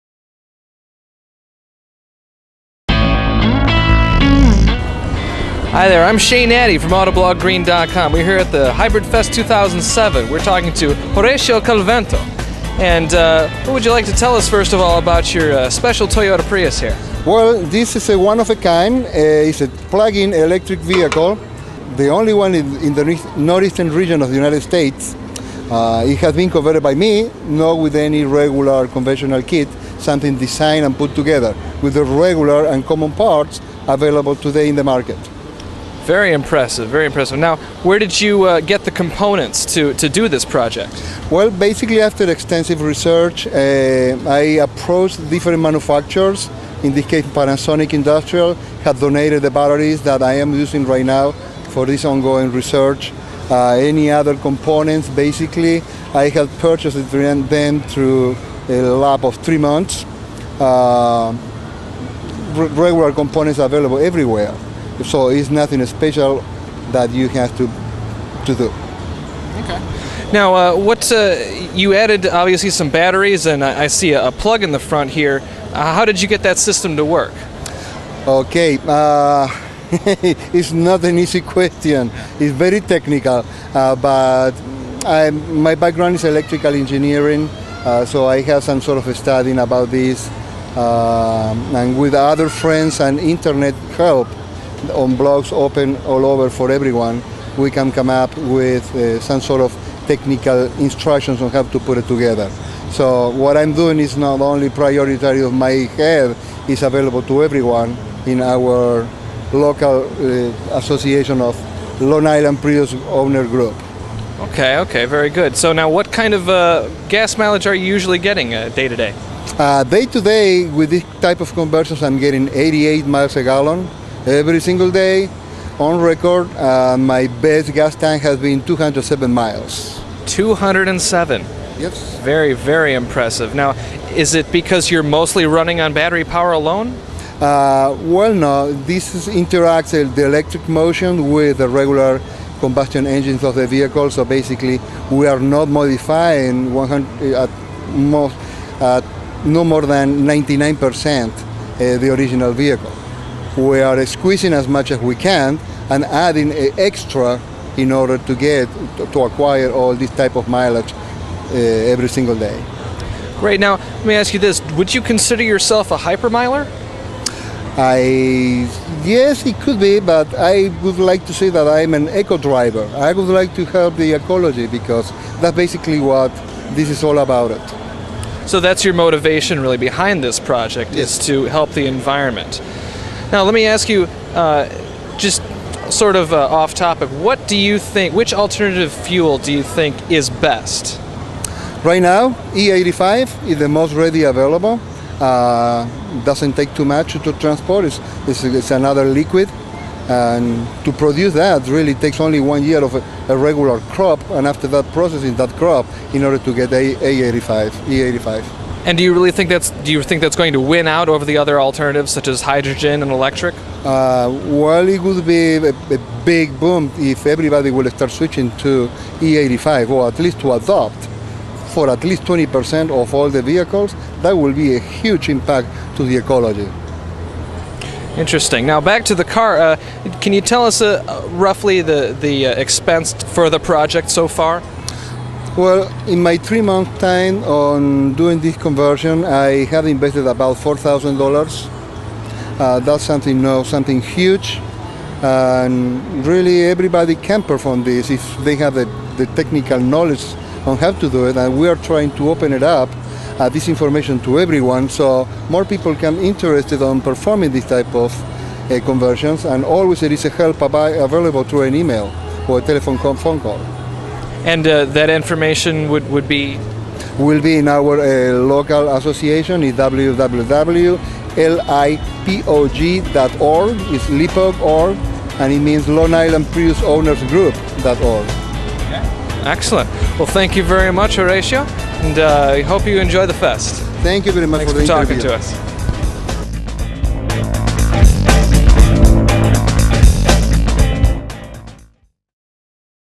Hybrid Fest 2007 VIDEO 5: Horacio Calvento's Plug-in Prius
Interview with Horacio Calvento from the east coast at Hybrid Fest 2007 in Madison, WI. Numerous modifications result in high mpg's - though not being a hypermiler - in a very factory-looking package. The trick: getting big companies like Panasonic to donate parts, and having the technical expertise to know how to put it all together.